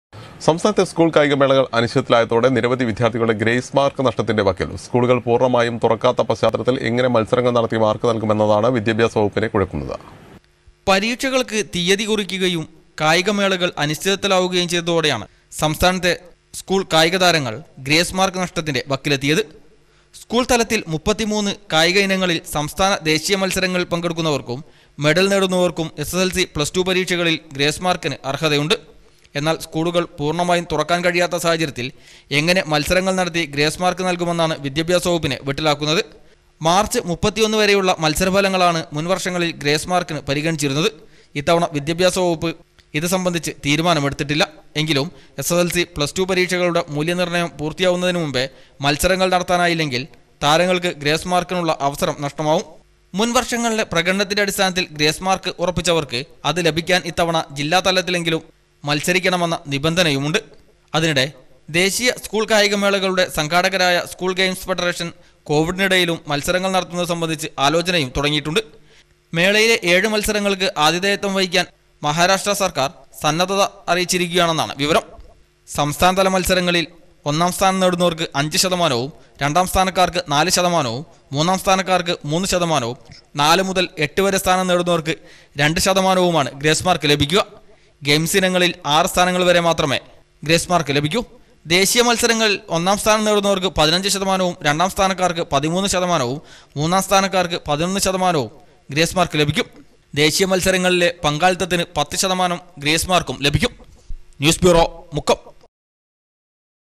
കായികമേളകൾ അനിശ്ചിതത്വത്തിലായതോടെ നിരവധി വിദ്യാർത്ഥികൾ ഗ്രേസ് മാർക്ക് നഷ്ടത്തിൻ്റെ വക്കിൽ .
സംസ്ഥാനത്തെ സ്കൂൾ കായികമേളകൾ അനിശ്ചിതത്വത്തിലായതോടെ നിരവധി വിദ്യാർത്ഥികൾ ഗ്രേസ് മാർക്ക് നഷ്ടത്തിൻ്റെ വക്കിൽ .സ്കൂളുകൾ പൂർണ്ണമായും തുറക്കാത്ത പശ്ചാത്തലത്തിൽ എങ്ങിനെ മത്സരങ്ങൾ നടത്തി മാർക്ക് നൽകുമെന്നതാണ് വിദ്യാഭ്യാസ വകുപ്പിനെ കുഴക്കുന്നത്.